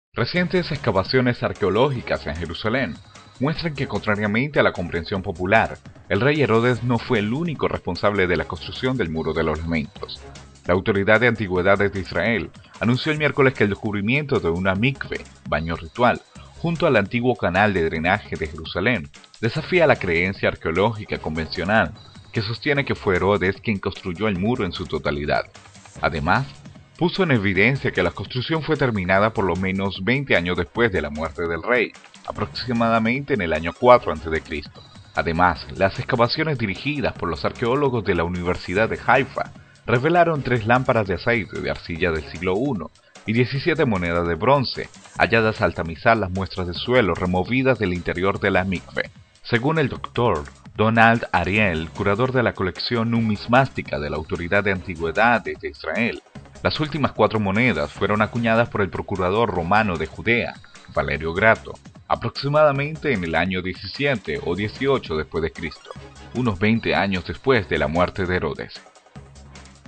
Excavaciones revelan que el rey Heródes no completo la construcción del Muro de los Lamentos
Recientes excavaciones arqueológicas en Jerusalén muestran que, contrariamente a la comprensión popular, el rey Herodes no fue el único responsable de la construcción del Muro de los Lamentos.
La Autoridad de Antigüedades de Israel anunció el miércoles que el descubrimiento de un mikve (baño ritual), junto al antiguo canal de drenaje de Jerusalén, desafía la creencia arqueológica convencional que sostiene que fue Herodes quien construyó el muro en su totalidad. Además, puso en evidencia que la construcción fue terminada por lo menos 20 años después de la muerte del rey, aproximadamente en el año 4 antes de Cristo.
Además, las excavaciones dirigidas por el arqueólogo Eli Shukron y el profesor Ronny Reich de la Universidad de Haifa, revelaron tres lámparas de aceite de arcilla del siglo I, y diecisiete monedas de bronce, halladas al tamizar las muestras de suelo removidas del interior de la mikve.
Según el Dr. Donald Ariel, curador de la colección numismática de la Autoridad  de Antigüedades de Israel, las últimas cuatro monedas fueron acuñadas por el procurador romano de Judea, Valerio Grato, aproximadamente en el año 17 o 18 después de Cristo, unos 20 años después de la muerte de Herodes.